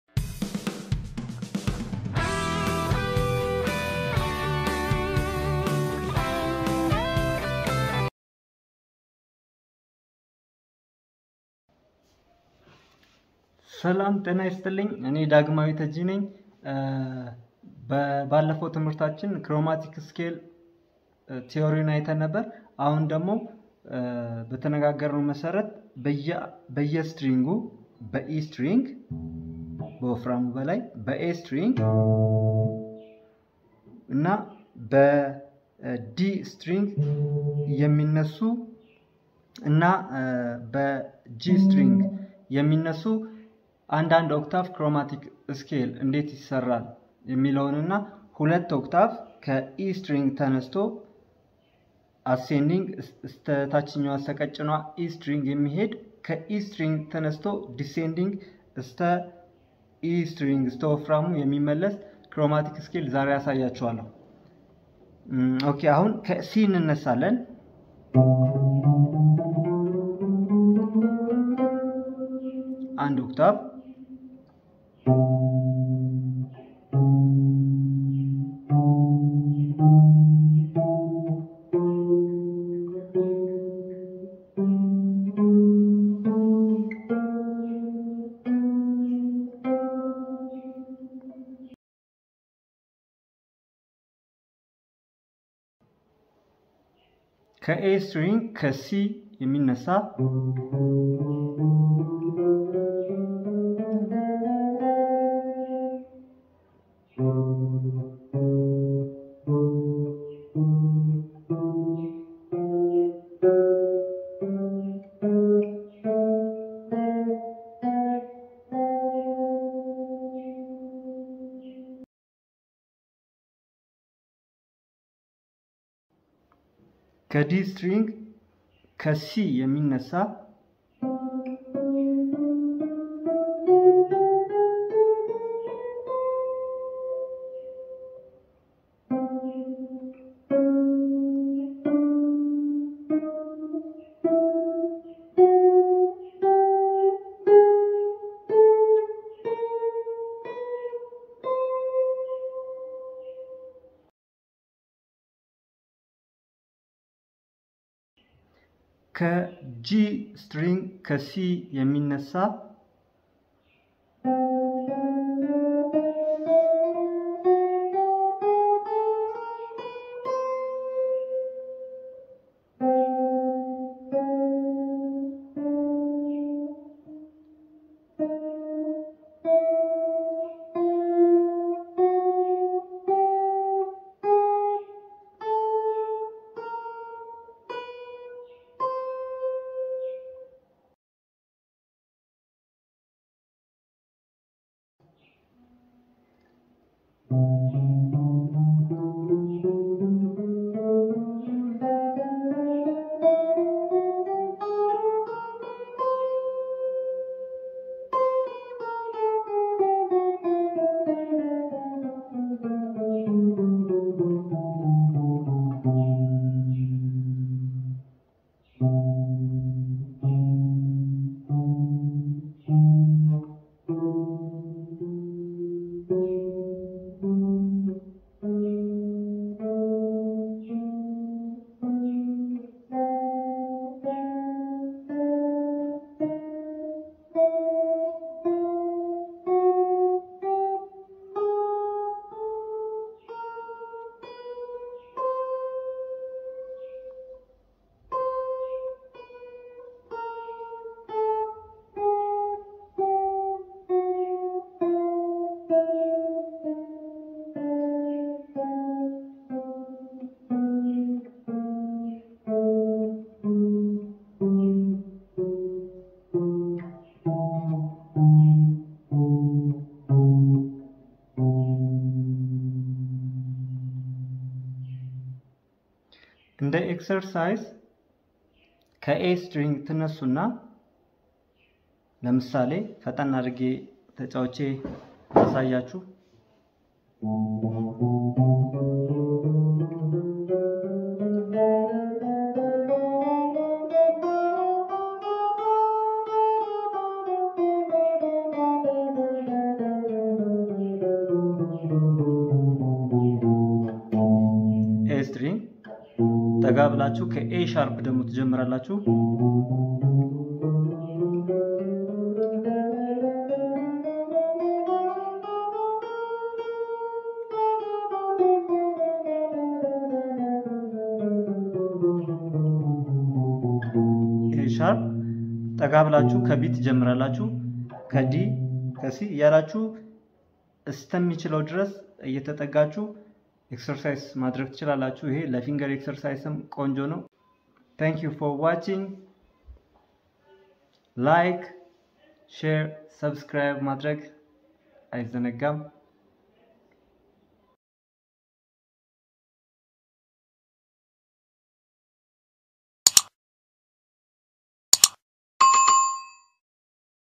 part 2 chromatic scale Amharic lesson
An octave scale which starts from each string. And two octave scales with ascending and descending order this two string octave scale can also be used for finger exercise. Starting from E string and A note you must work on each key to improve your finger adroitness.